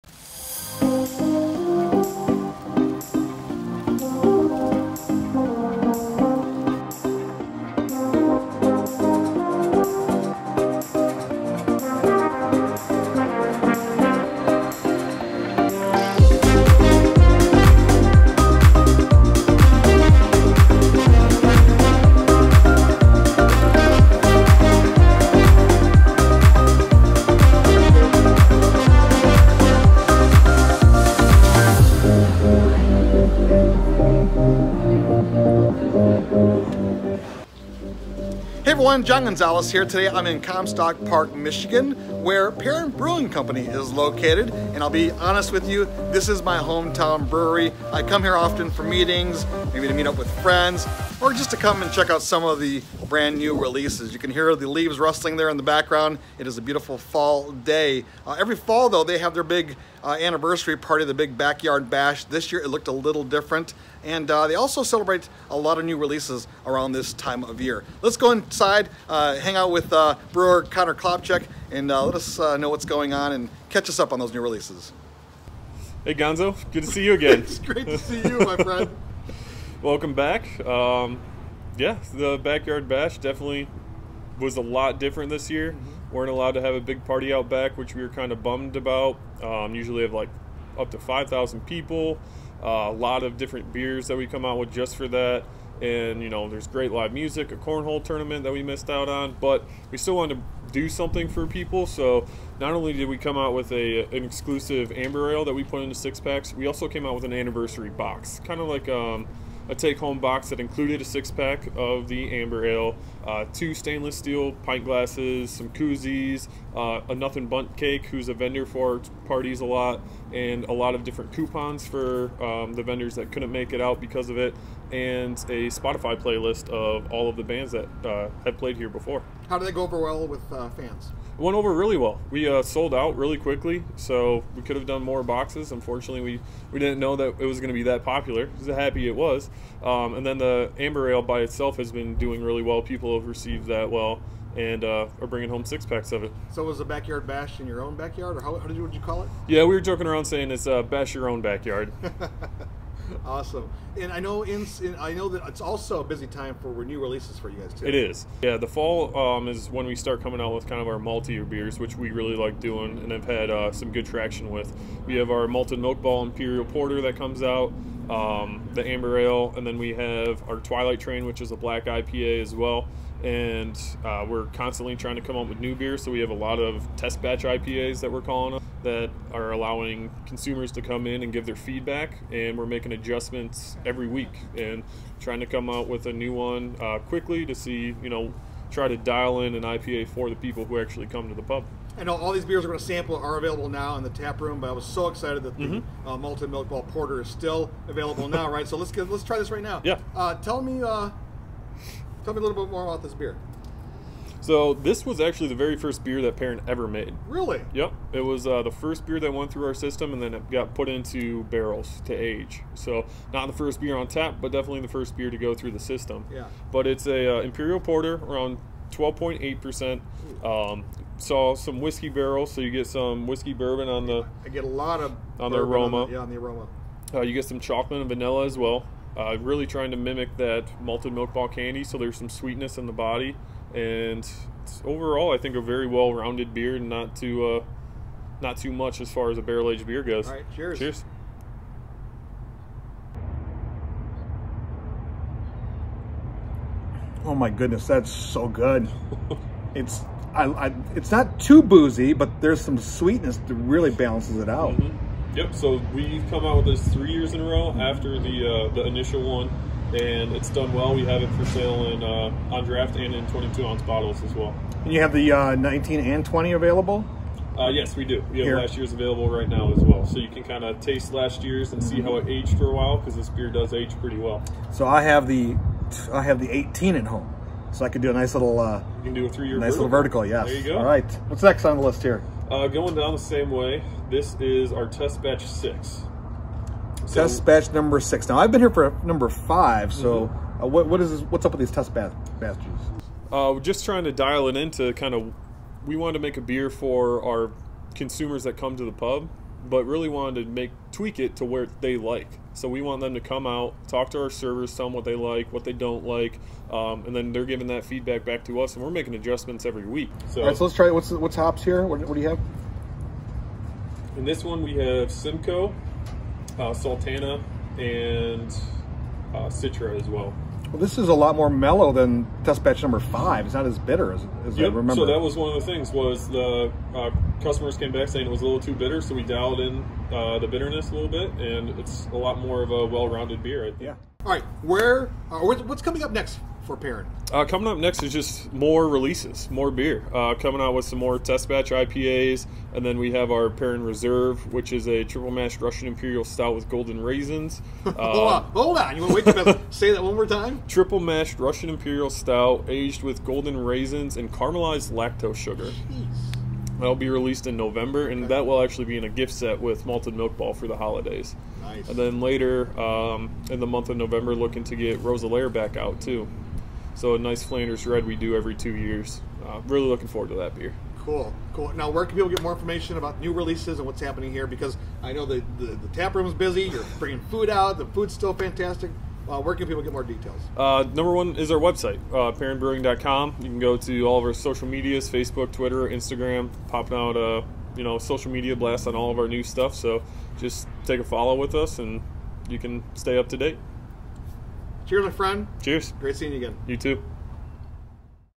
Perrin Brewing Company celebrates 8th anniversary with new releases
Perrin Brewing Company in Comstock Park, near Grand Rapids, typically has a backyard bash with 5,000 fans to celebrate its anniversary. This year because of the pandemic, they celebrated their 8th anniversary with gift boxes for customers to bash their own backyard. In this interview with MLive's John Gonzalez, Connor Klopcic, Perrin's director of brewing operations, said the pandemic has not stopped them from brewing up their favorite beers, including fan favorite the Malted Milk Ball Imperial Porter. Learn more about Perrin at perringbrewing.com.